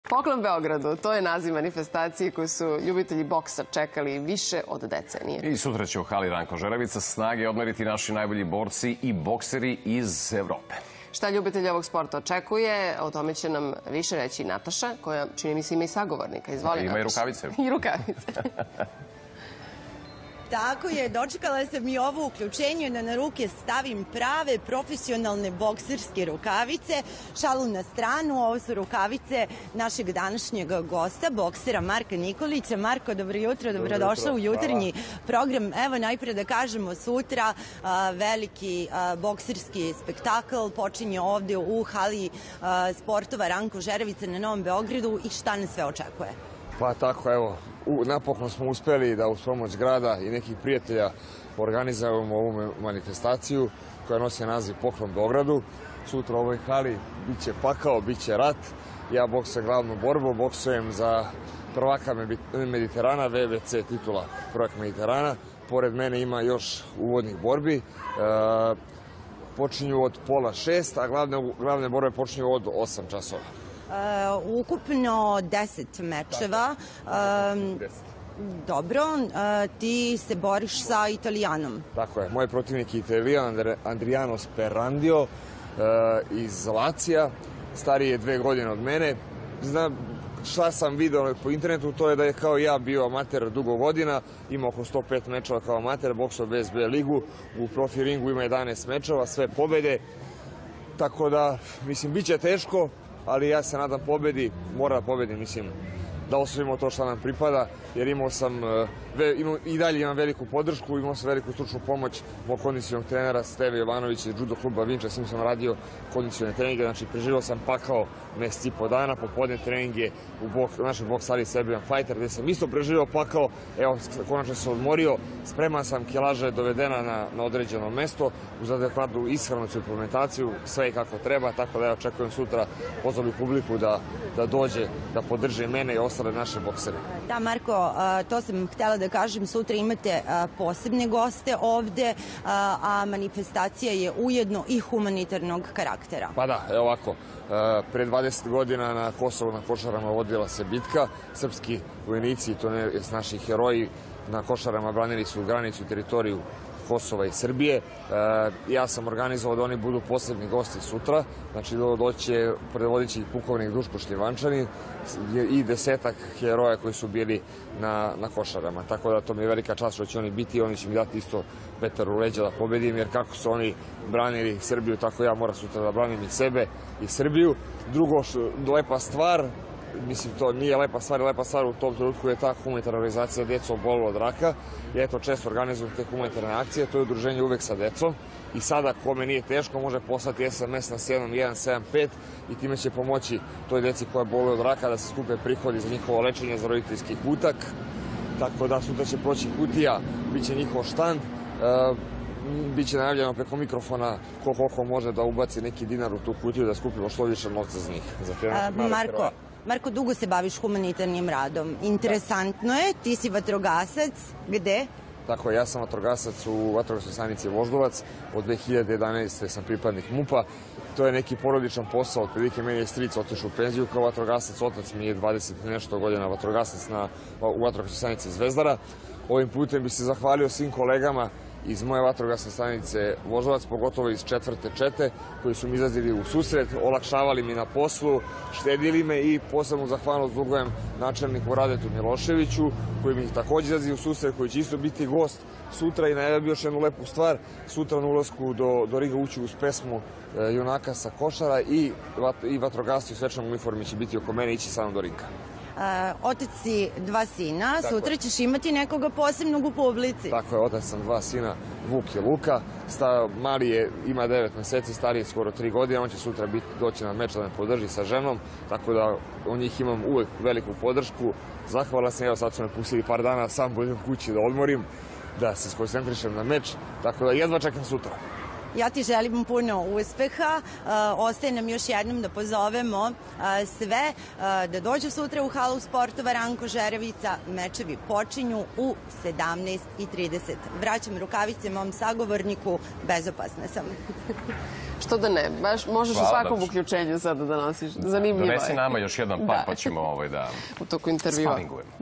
Bokseri na Poklon Beogradu
''Poklon Beogradu'' naziv je manifestacije koju su ljubitelji boksa čekali više od decenije. Sutra će u hali "Ranko Žeravica" snage odmeriti naši najbolji borci i bokseri iz Evrope.